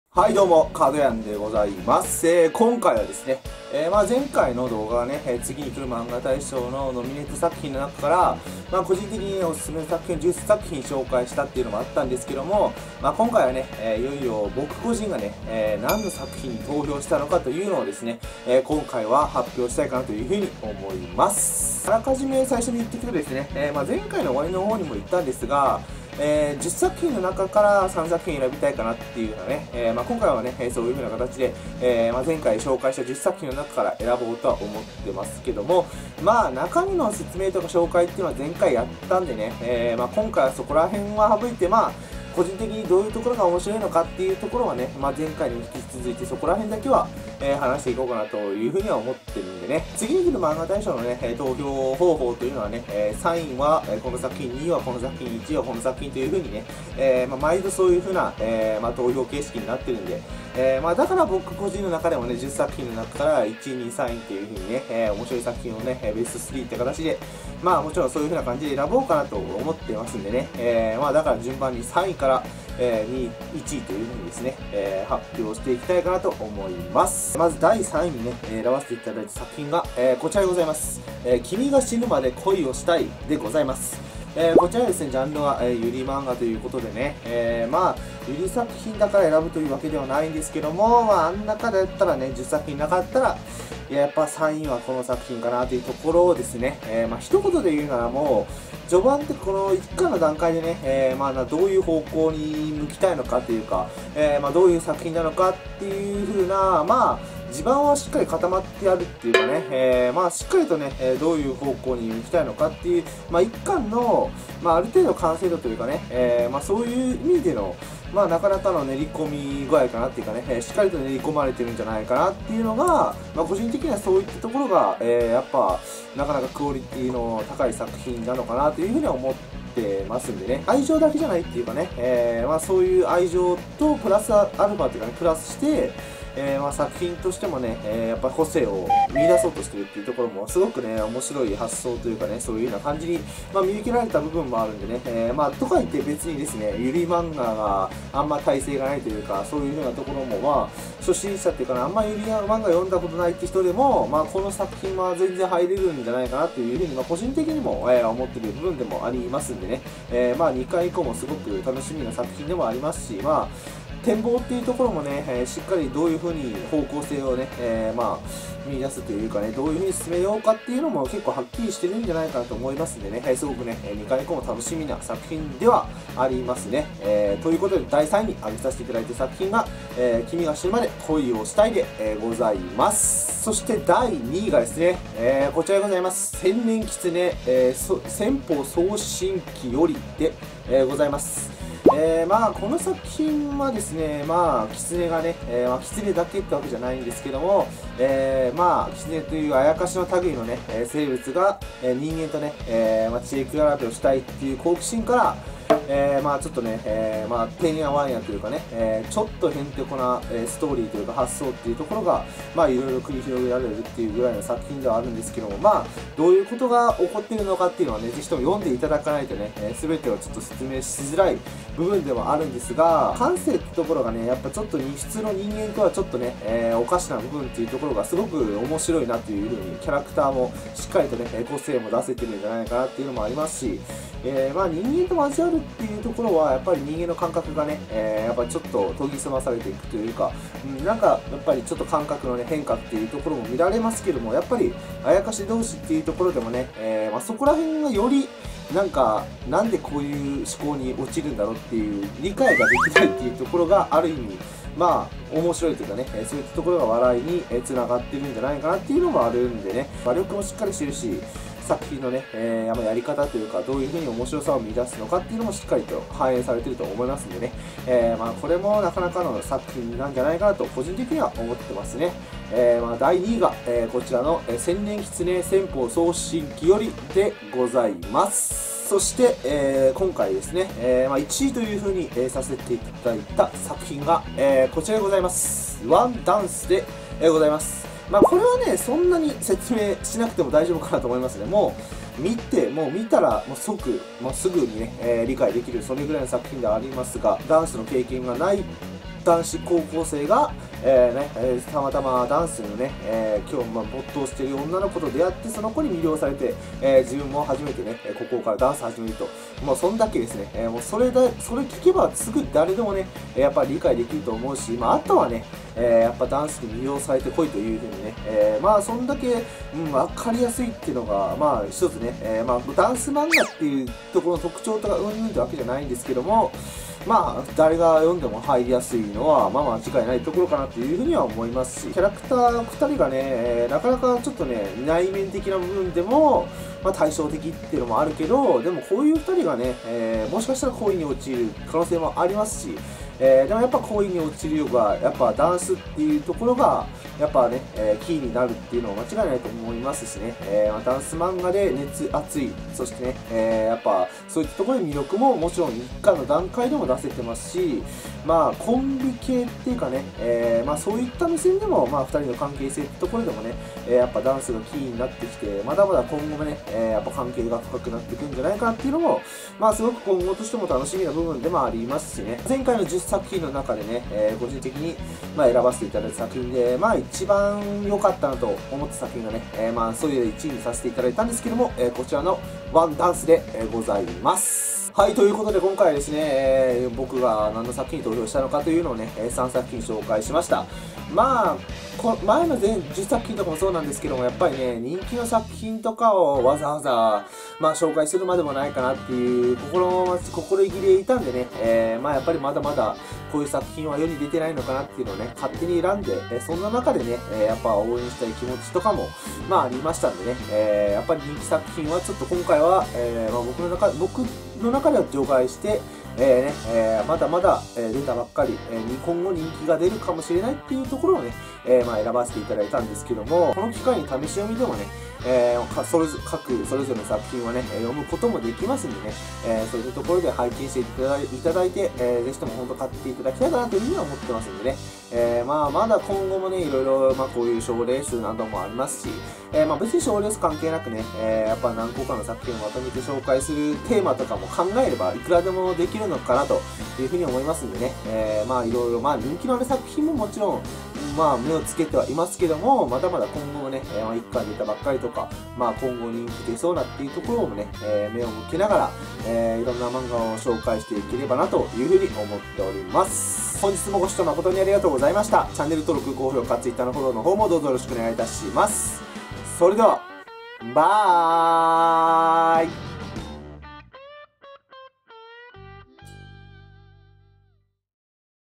次にくるマンガ大賞２０１９ かどやんが投票した作品を発表！
僕が投票した３作品を発表しました！
すでに人気だと思う作品は除外したうえで選んでいるので何が１番面白いのかは決め切れないです。
みなさんのおすすめ作品或いは投票した作品も宜しければ教えてください！